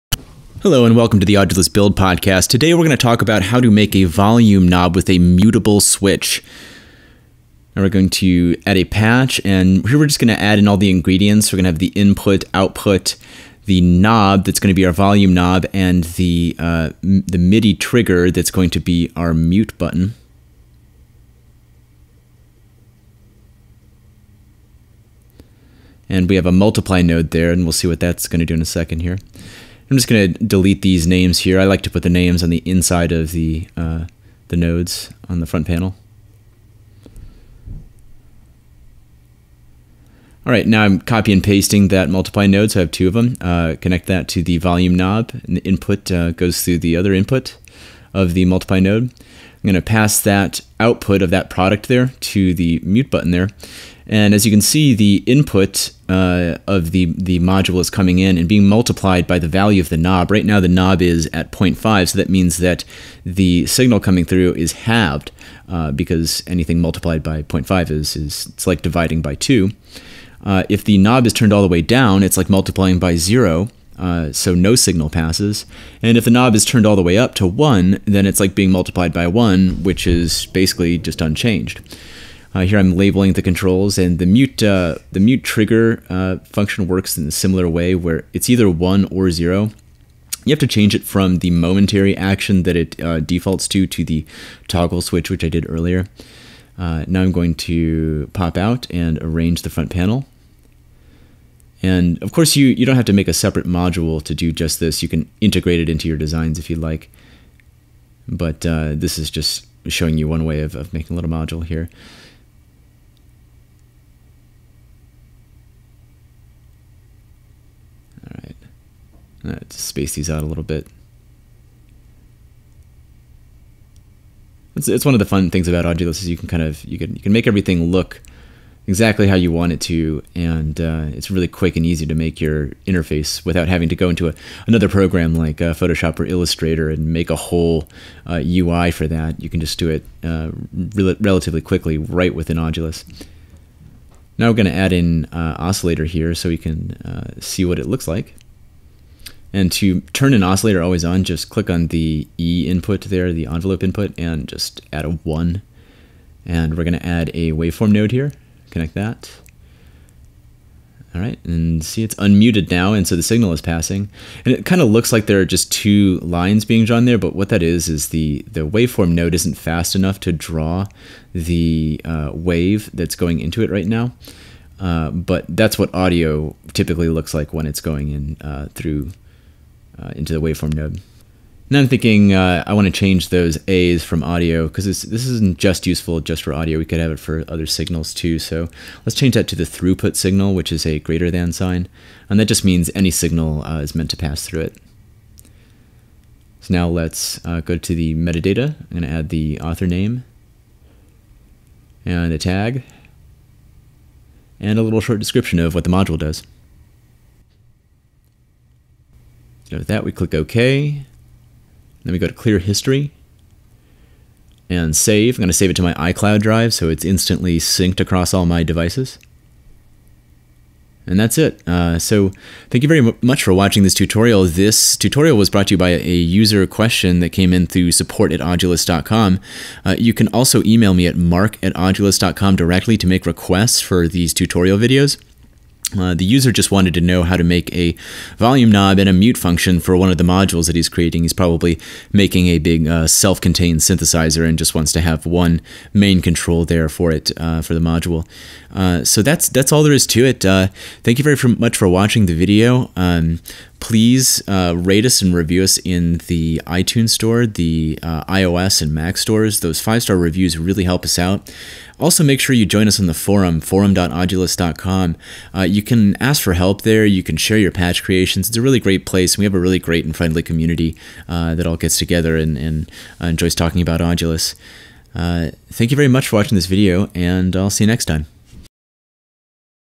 Audulus Build Podcast #4: Volume Knob with Mute
The Audulus Build Podcast hosted by Bimini Road explains how to approach building your own custom synthesizer modules with Audulus.

Note: This series assumes familiarity with the menus and basic functions of Audulus and modular synthesis in general.  If you need a more basic introduction to Audulus, check out the introductory tutorials

This episode Mark shows you how to make a volume knob with a toggleable mute.